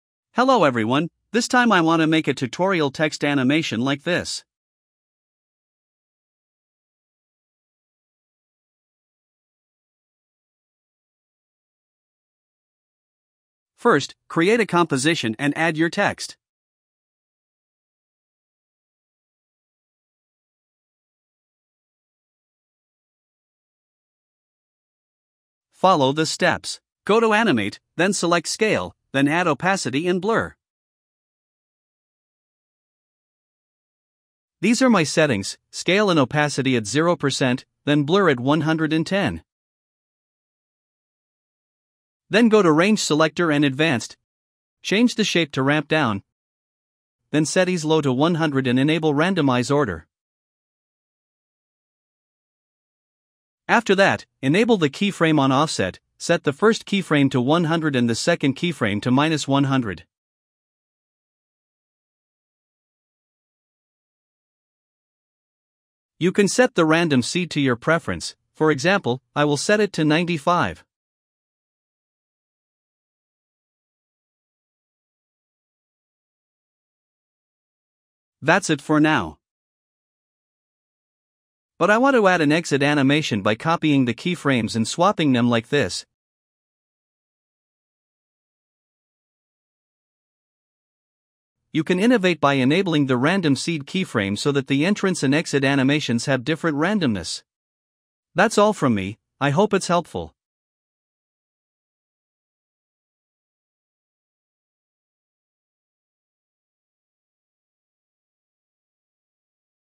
How to Animate Blur & Scale Text in After Effects | After Effects Tutorial
Learn how to create a stylish random blur and scale text animation in After Effects! This easy tutorial uses only built-in tools no plugins needed. Perfect for adding dynamic text animations for intros, presentations, or end credits.

📌 Timestamps
00:00 Intro
00:11 Tutorial
01:23 Done
01:32 Notes